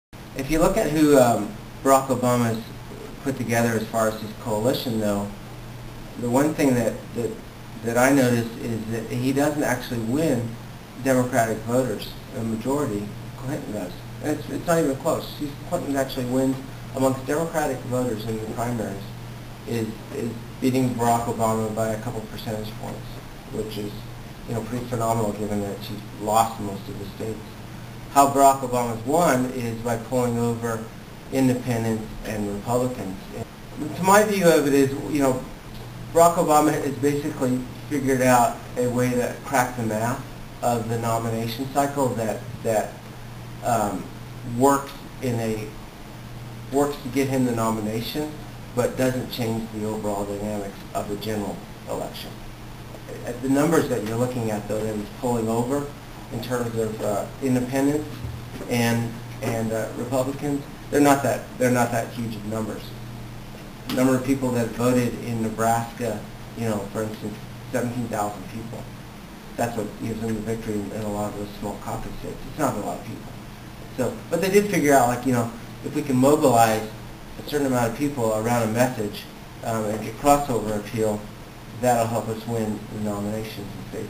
Jerome Armstrong: Obama "Cracked The Math" Of Dem Nomination
On March 3, 2008, Jerome Armstrong (founder of MyDD.com and co-author of Crashing the Gate) spoke to the Internet & Politics class at George Washington University.

In this clip he says that Clinton is winning Democrats, but is losing the Democratic nomination because Obama "cracked the math of the nomination cycle" and figured out how to bring over just enough Independents to win more delegates. But, he says, Obama is not actually winning massive amounts of Independents—the number needed to win the Nebraska caucus (17,000), for instance, was very small from a quantity perspective.